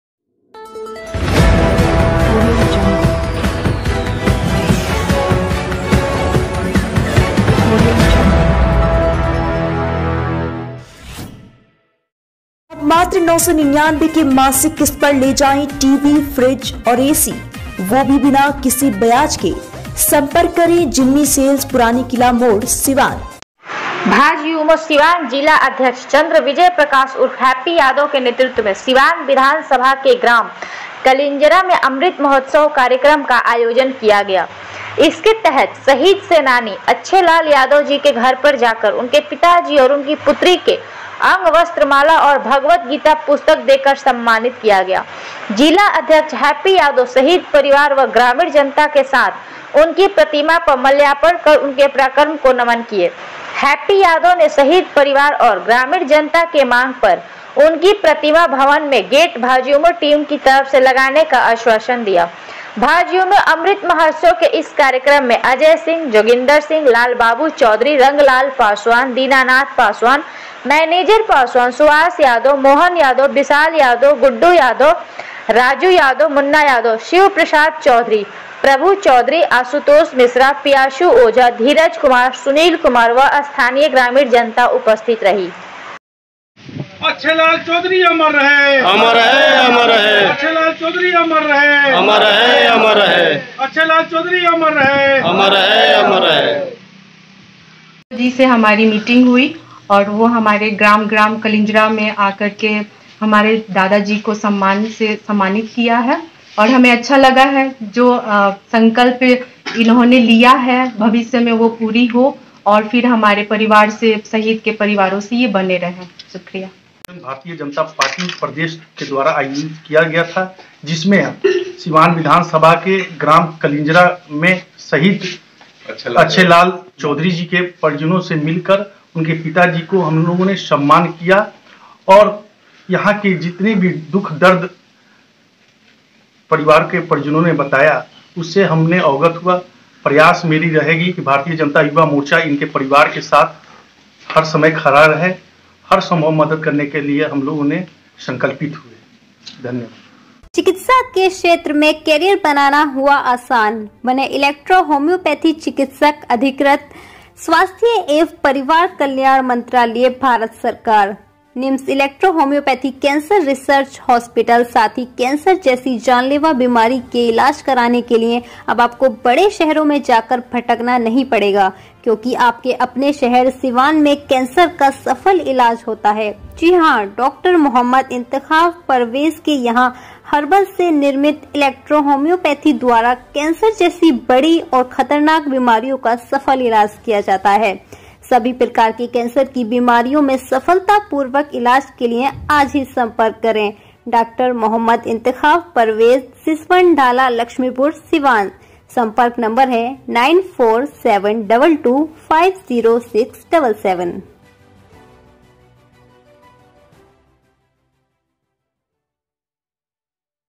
सिवान- भाजयुमों ने शहीद परिवार को किया सम्मानित, किया ये बड़ा वादा...
भाजयुमो सीवान जिलाध्यक्ष चन्द्र विजय प्रकाश उर्फ हैप्पी यादव के नेतृत्व में सीवान विधानसभा के ग्राम कलींजरा मे अमृत महोत्सव कार्यक्रम तहत शहीद सेनानी अच्छे लाल यादव जी के पर जाकर उनकें पिताजी और उनकी पुत्री को अंगवस्त्र, माला और भगवत गीता पुस्तक देकर सम्मानित कियें। जिलाध्यक्ष हैप्पी यादव शहीद परिवार व ग्रामीण जनता के साथ उनकी प्रतिमा पर माल्यार्पण कर उनके पराक्रम को नमन कियें। हैप्पी यादव ने शहीद परिवार और ग्रामीण जनता के मांग पर उनकी प्रतिमा भवन मे गेट भाजयुमो टीम की तरफ से लगाने का आश्वासन दियें। भाजयुमो अमृत महोत्सव के इस कार्यक्रम मे अजय सिंह भावी मुखिया प्रत्याशी, जोगिंदर सिंह,लालबाबू चौधरी, रंगलाल पासवान, दीनानाथ पासवान, मैनेजर पासवान, सुभाष यादव,मोहन यादव,विशाल यादव,गड्डू यादव,राजू यादव,मुन्ना यादव,शिवप्रसाद चौधरी,प्रभू चौधरी,आशुतोष मिश्रा, पियाषू ओझा,धीरज कुमार, सुनील कुमार व स्थानीय ग्रामीण जनता उपस्थित रहीं।